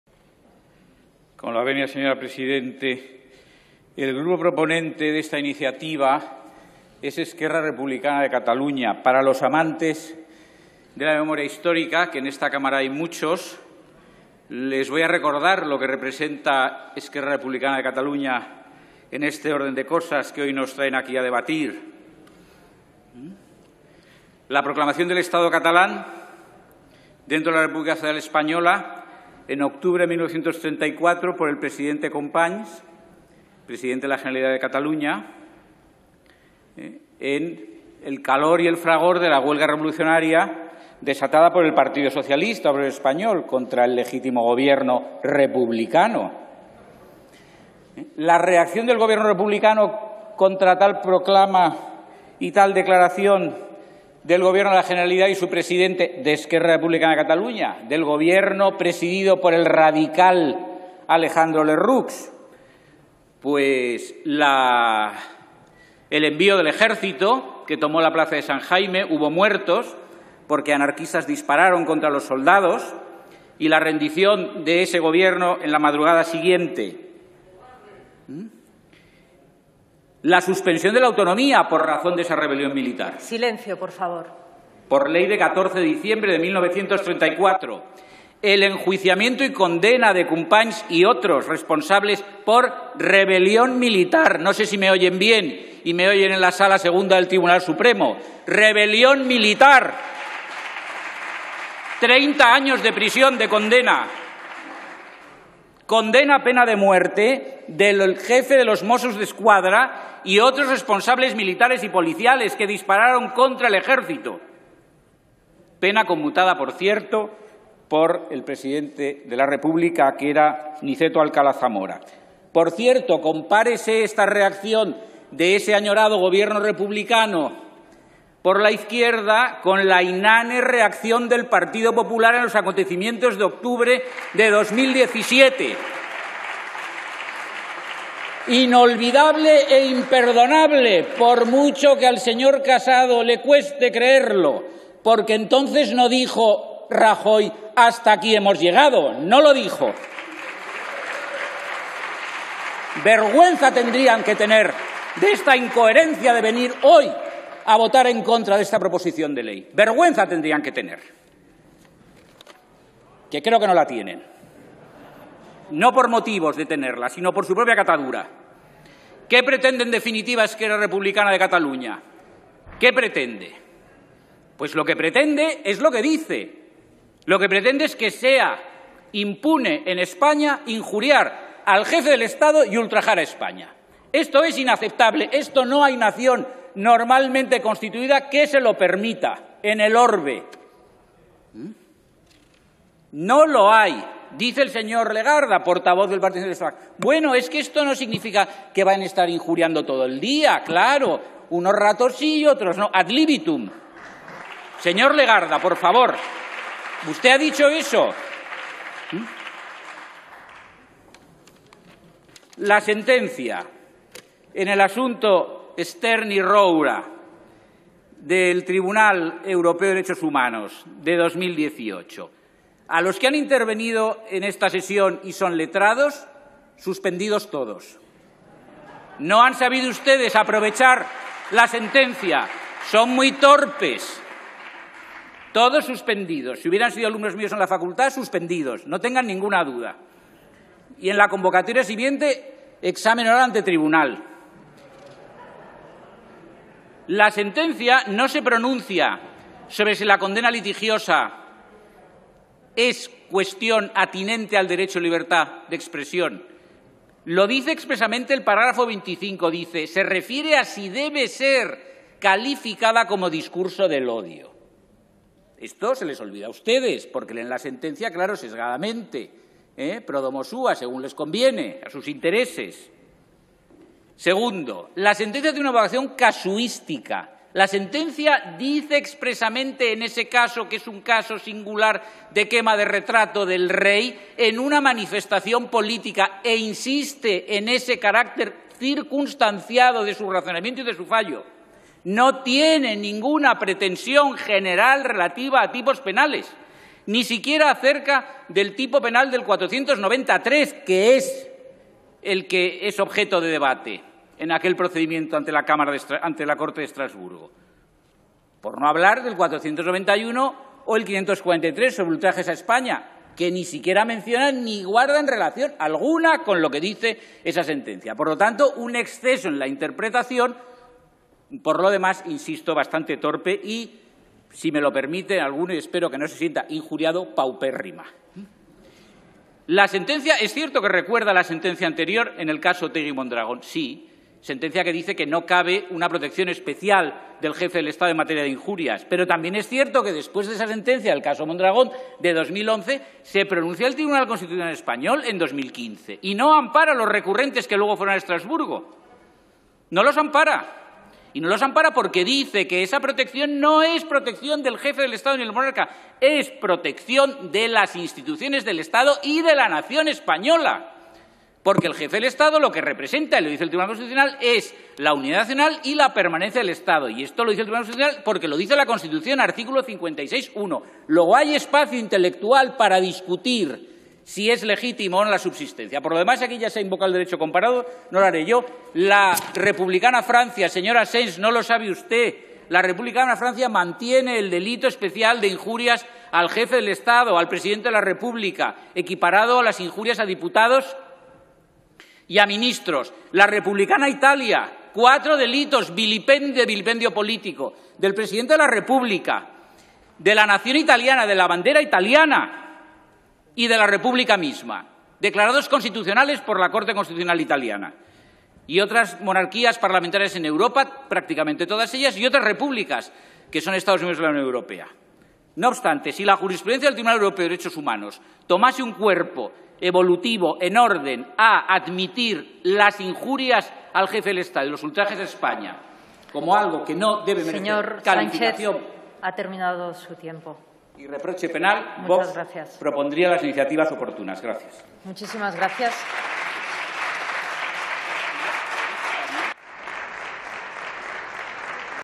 Intervención de José María Sánchez hoy 27 de Octubre 2020 sobre despenalización injurias a la Corona
Intervención de José María Sánchez García de VOX, sobre la Proposición de Ley por la que se modifica la Ley Orgánica 10/1995, de 23 de noviembre, del Código Penal, para despenalizar las injurias a la Corona y los ultrajes a España, el día 27 de Octubre de 2020.